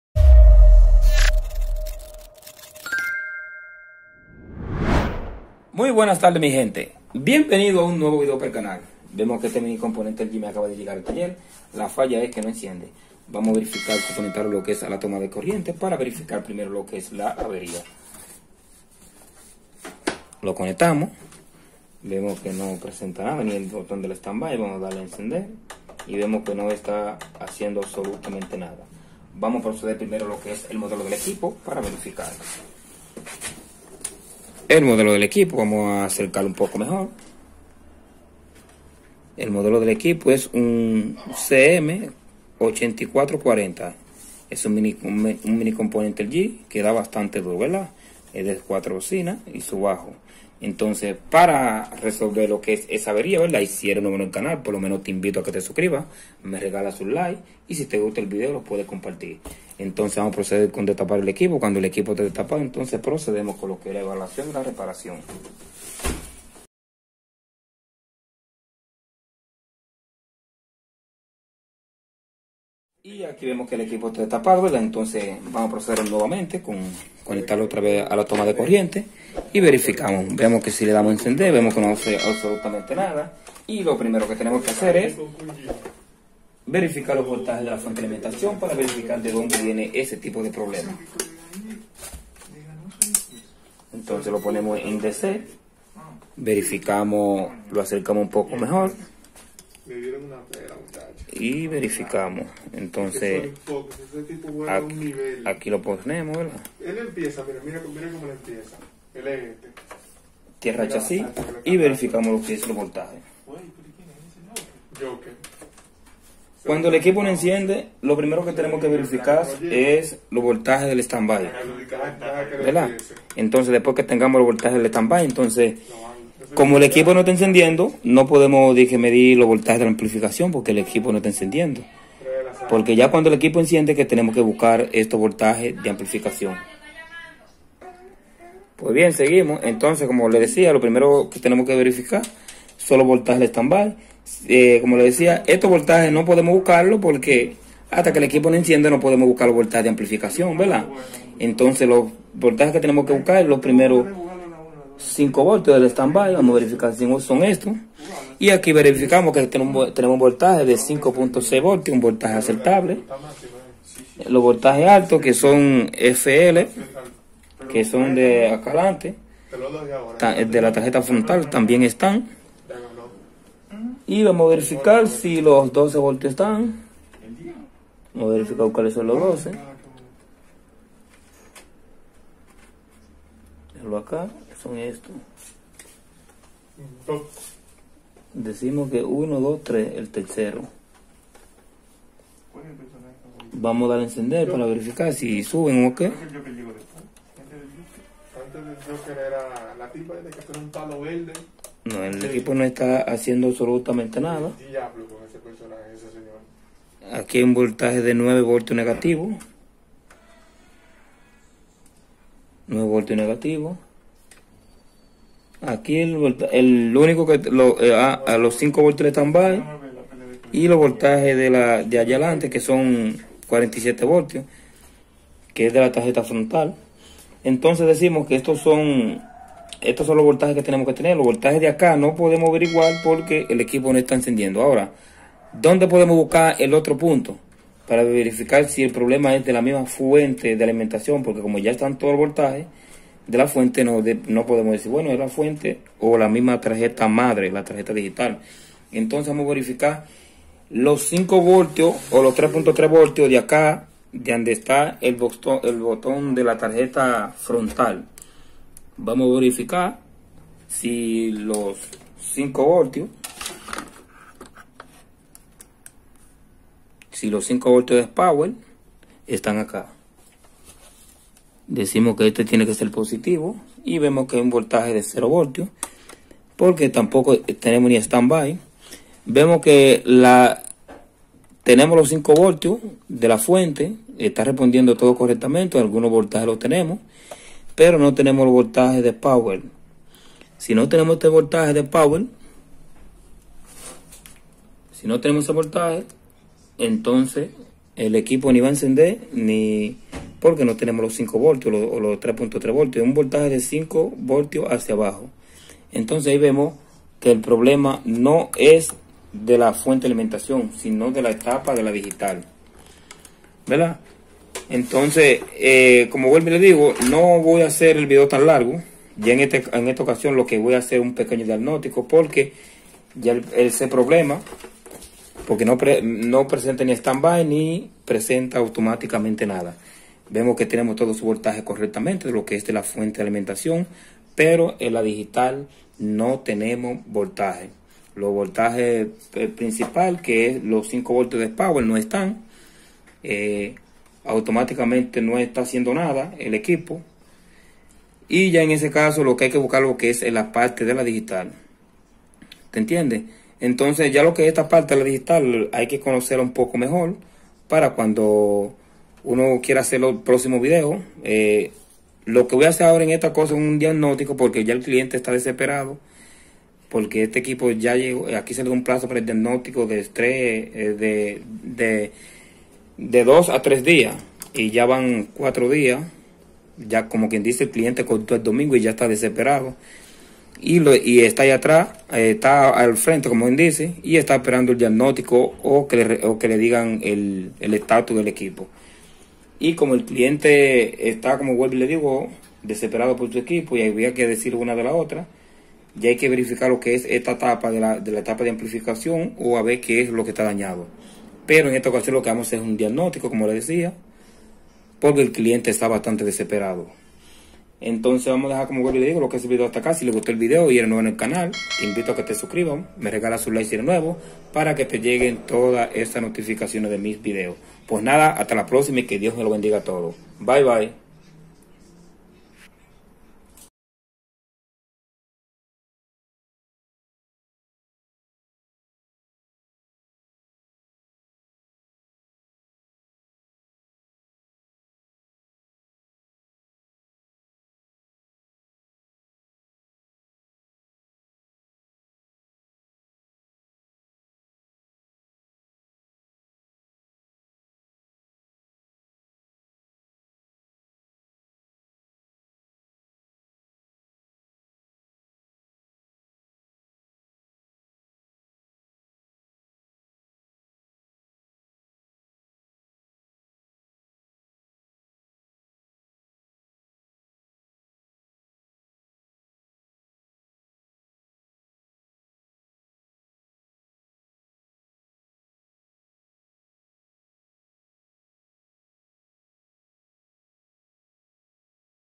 MINICOMPONENTE LG NO ENCIENDE DIAGNÓSTICO MODEL.CM8440
EN ESTE VIDEO LE ENCEÑAREMO A COMO DIAGNÓSTICAR EL MINICOMPONENTE LG QUE NO ENCIENDE